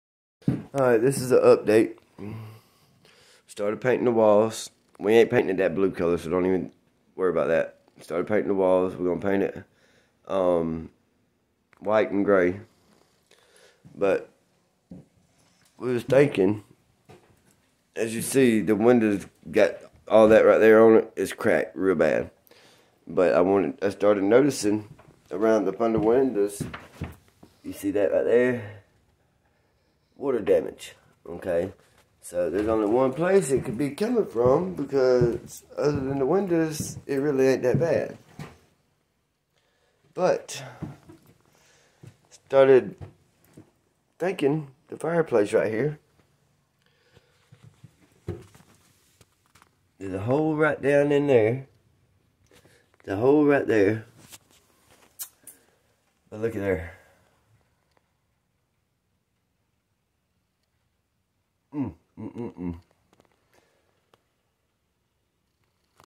mold and water damage repair
was wanting to do the floors take the carpet up and put the wood down started noticing water damage whenever pulled up the carpet up under the windows had to come from somewhere so it started looking and as you see behind the fireplace all kind of water damage because people didn't know how to put the cap back on the chimney whenever they done the metal roof